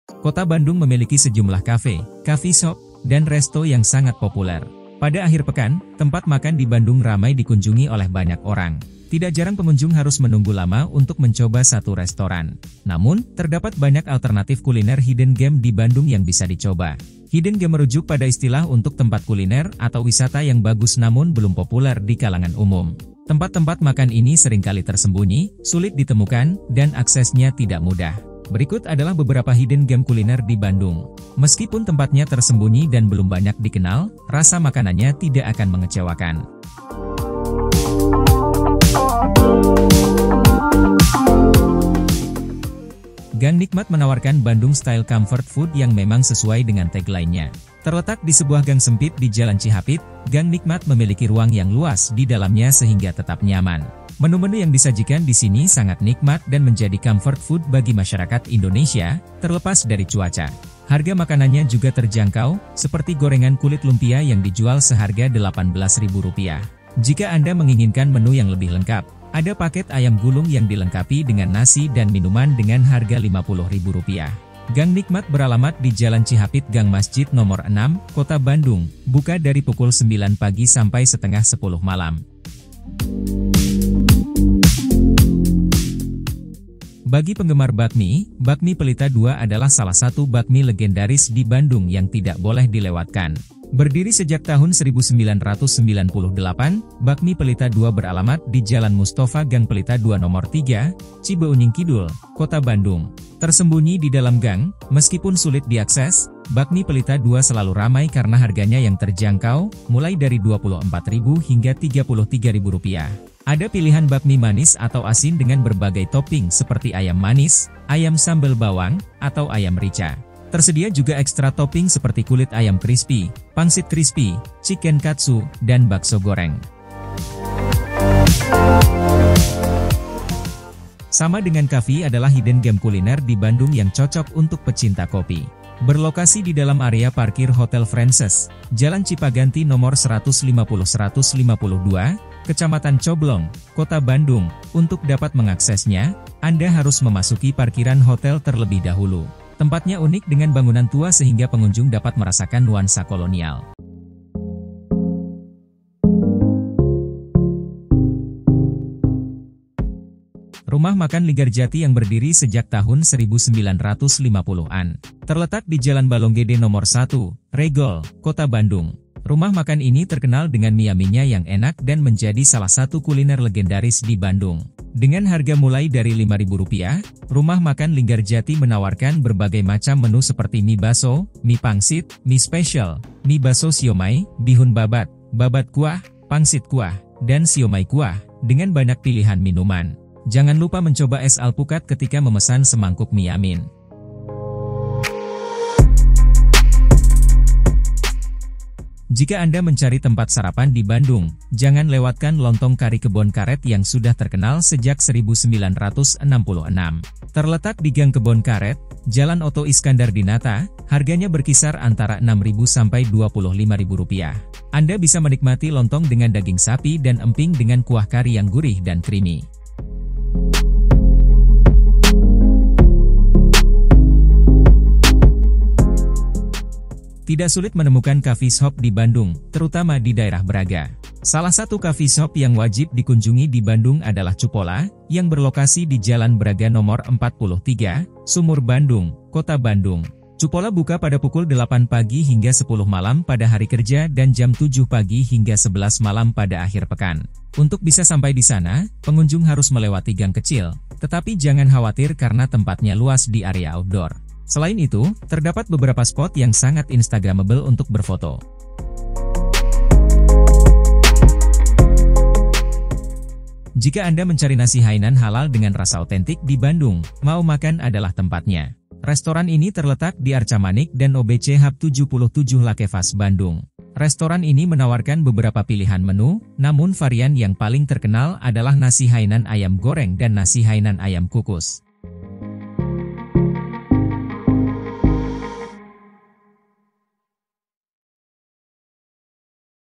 Gang Sempit, Rasa Bintang 5! Kuliner Hidden Gems di Bandung Ini Bikin Ketagihan
Gang sempit, rasa bintang 5? 🤯 Bandung memang surganya kuliner hidden gems! Kali ini, kita akan ajak kamu kulineran ke tempat makan legendaris yang tersembunyi di gang-gang sempit Bandung. Jangan remehkan lokasinya, karena rasa makanan di sini dijamin bikin kamu ketagihan! 😋

Penasaran di mana saja tempat makan hidden gems di Bandung yang wajib kamu coba? Simak video ini sampai habis dan temukan rekomendasi kuliner Bandung paling otentik dan lezat! 😍 Dijamin, kamu gak akan nyesel! 😉